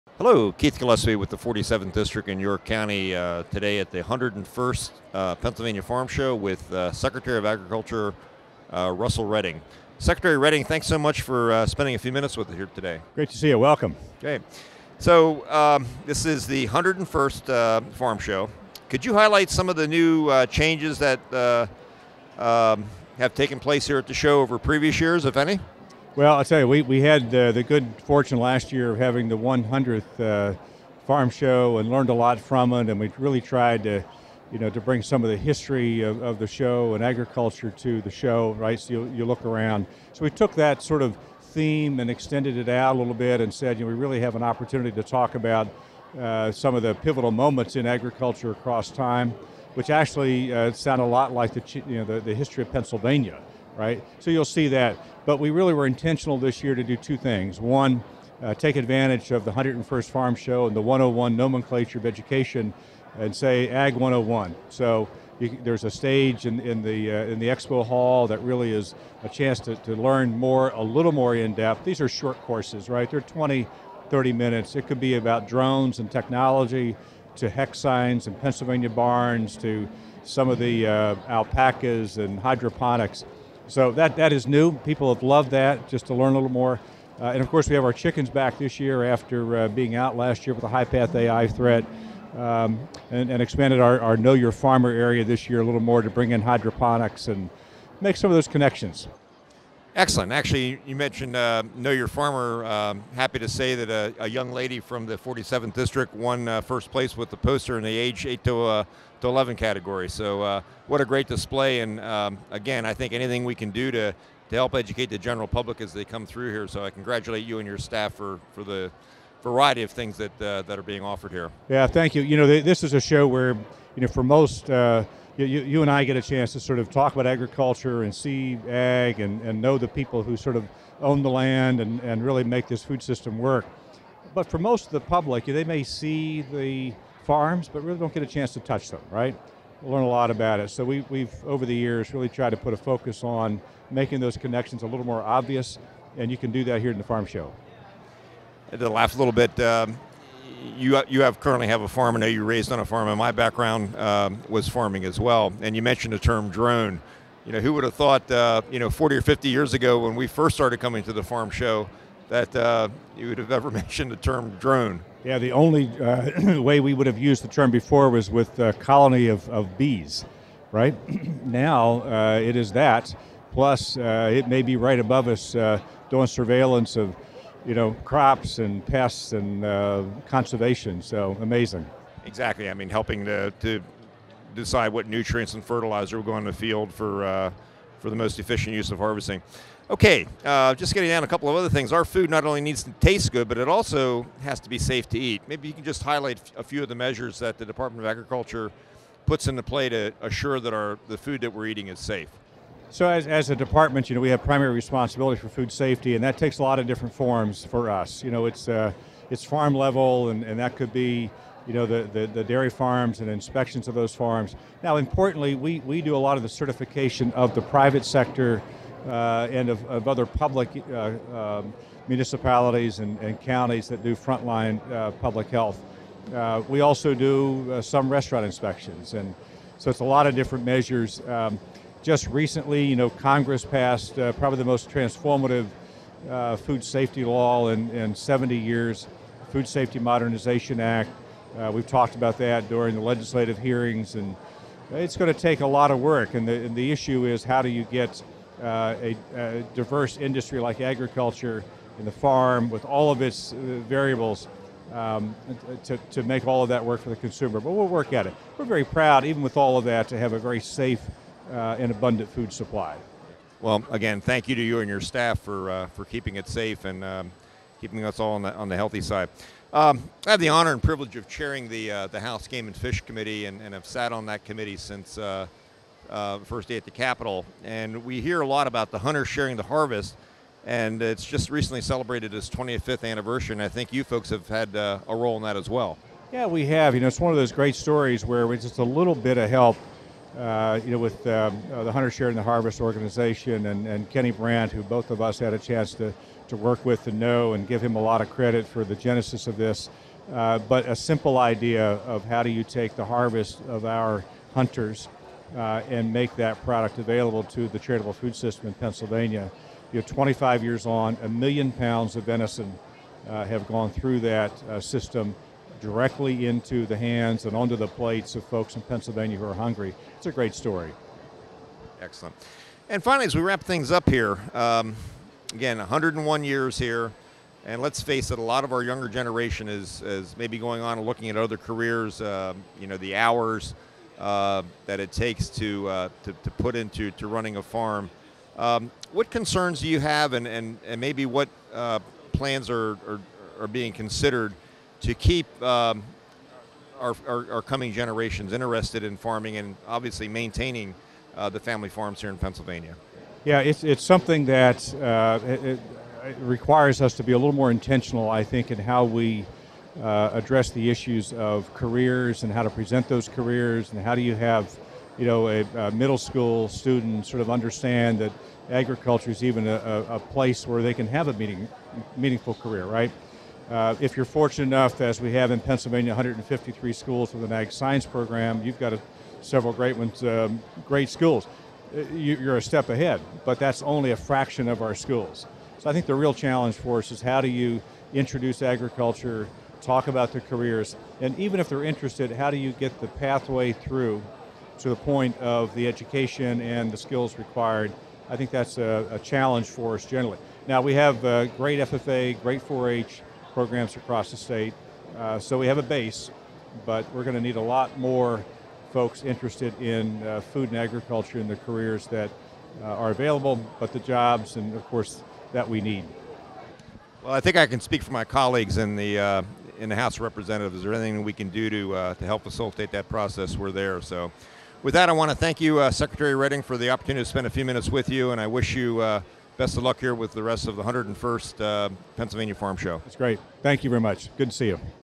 Interview with PA Ag Secretary Redding
Pa. State Rep. Keith Gillespie interviews Pa. Secretary of Agriculture Russell Redding at Pennsylvania's 101st Farm Show.